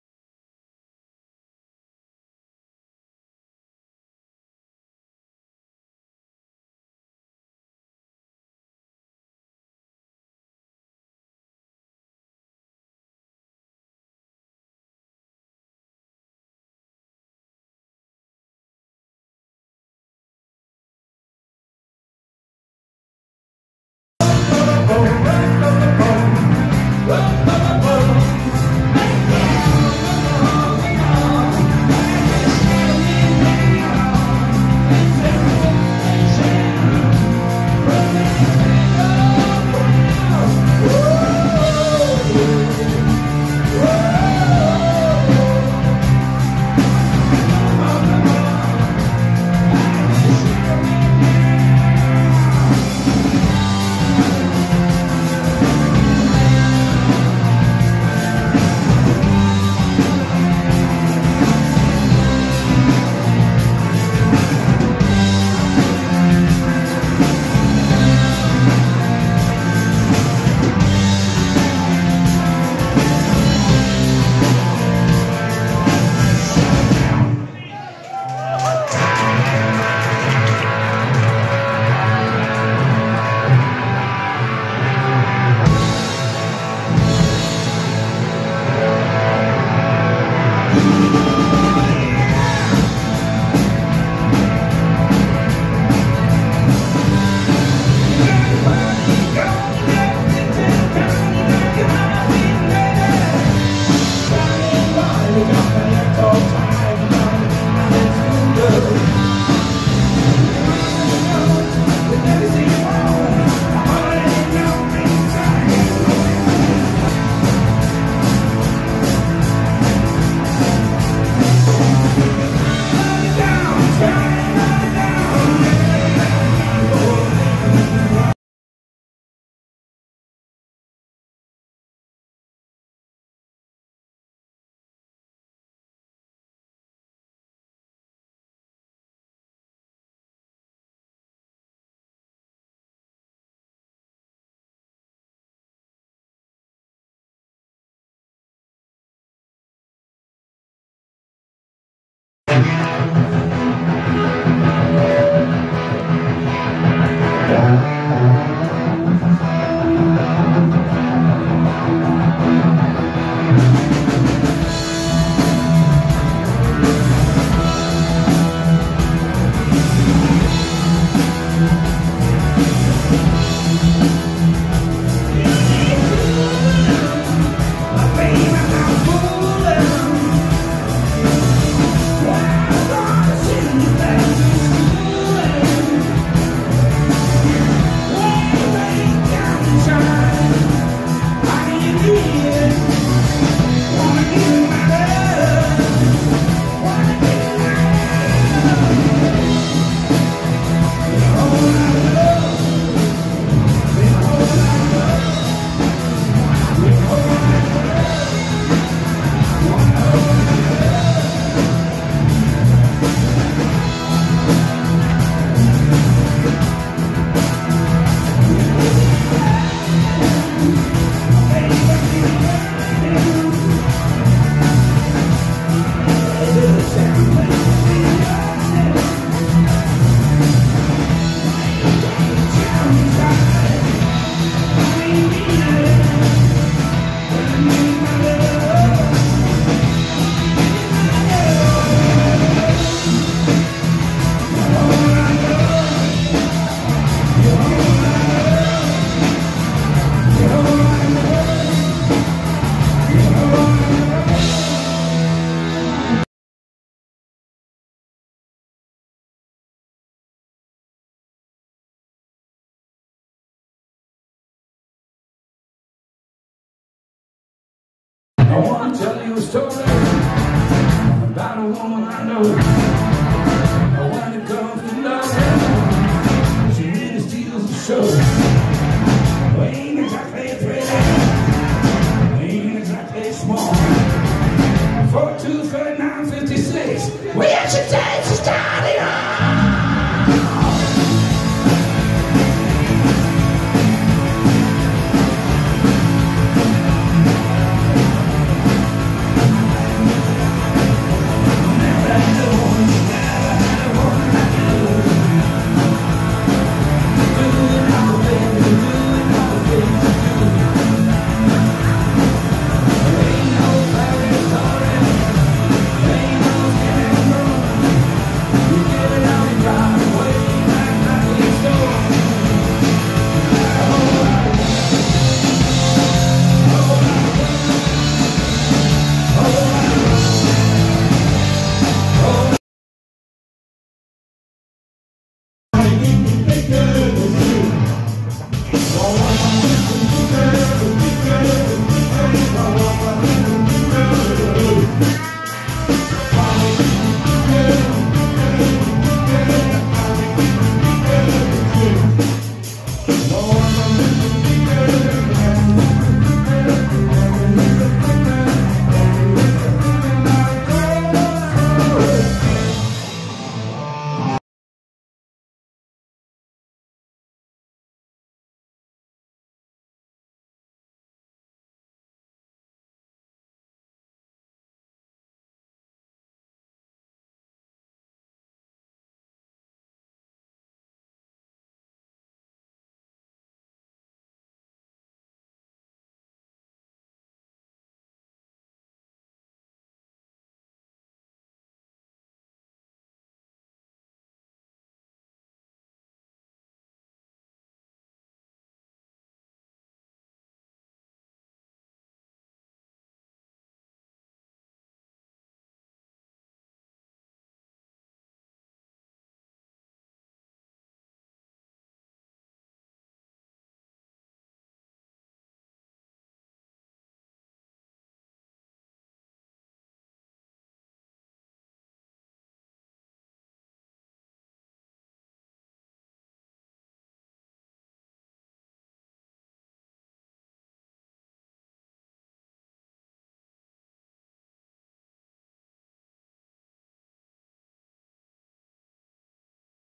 RIPDEAL LIVE 2024 Bagatel Belgium COMPILATION7
Covers of:

Rammstein
Pink Floyd
Golden Earring
Triggerfinger
Gnarls Barkley
De Kreuners
Anouk
The Cranberries
AC/DC
Rihanna
Tina Turner
The Cure
Goldband
Matthias Reim
Judas Priest
The Scene
Queen